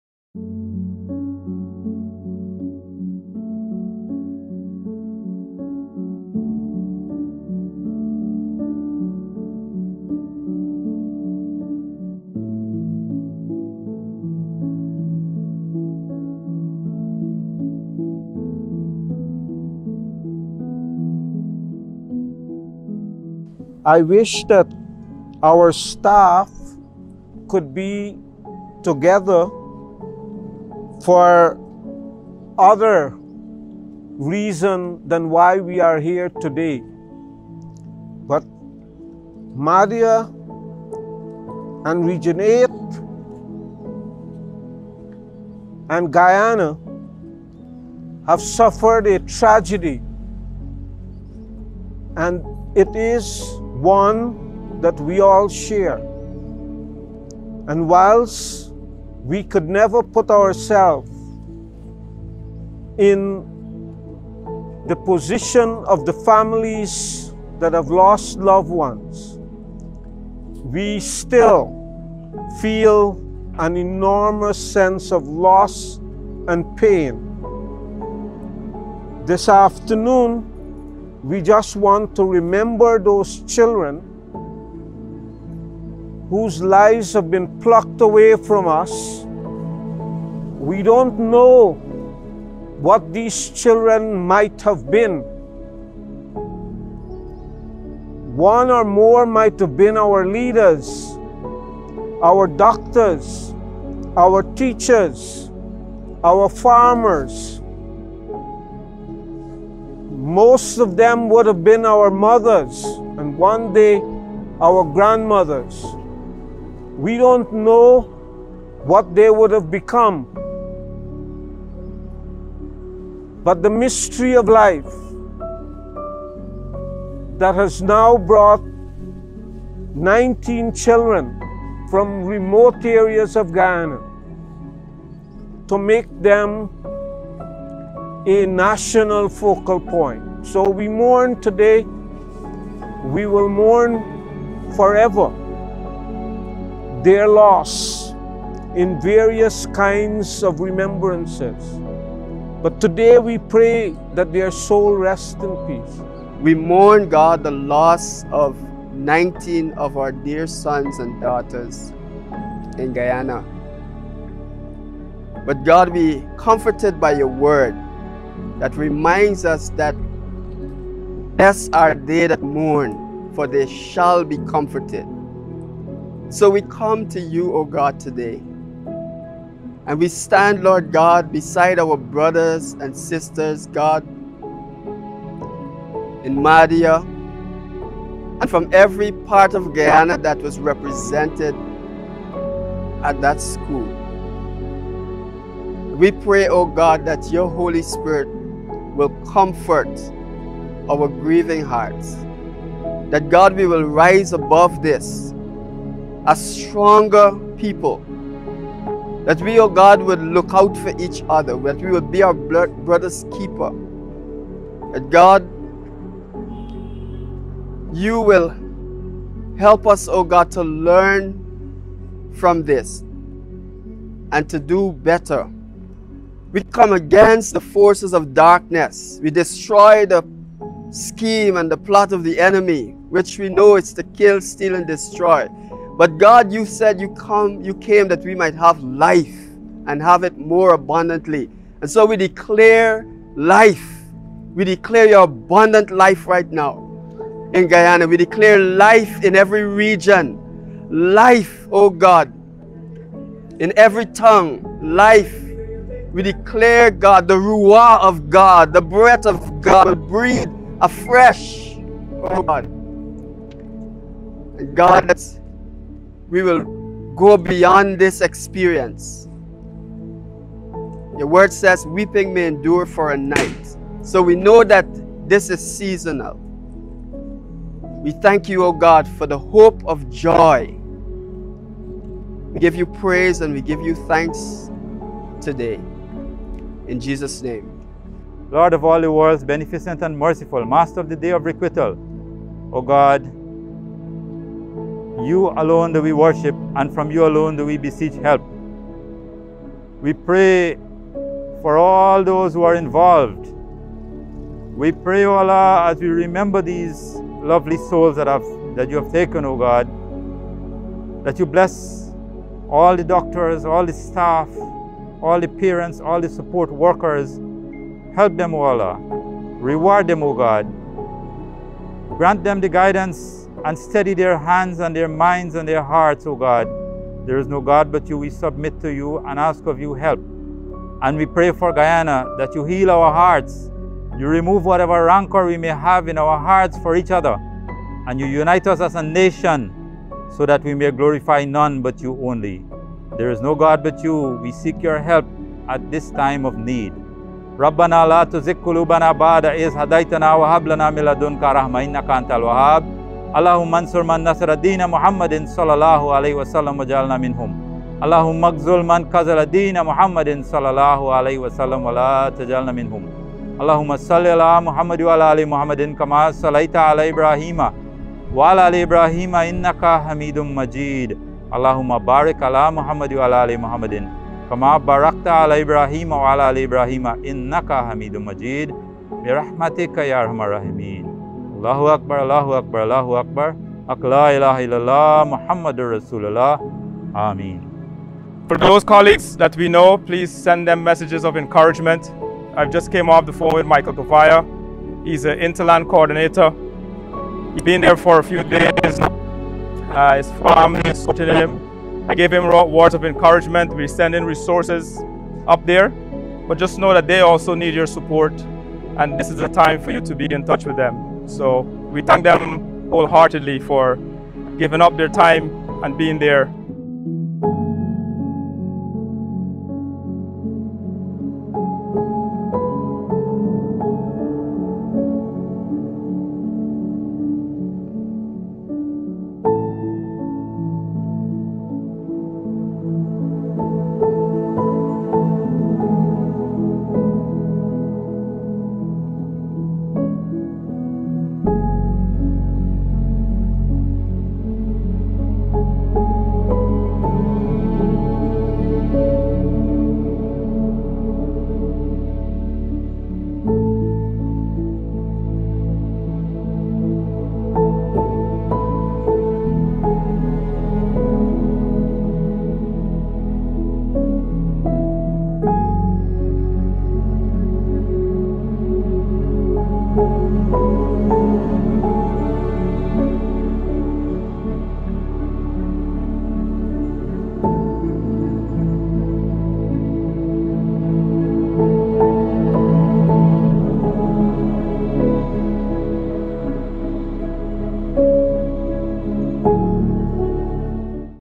Moment of Silence
On Thursday, Advisors to the Minister of Health Dr Lesile Ramsammy and Dr Bheri Ramsaran; Permanent Secretary, Mr Malcolm Watkins and other staff of the Ministry of Health, gathered in the Ministry‘s compound on Brickdam to join in prayers and observe a moment of silence for the lives lost in the tragic Mahdia fire.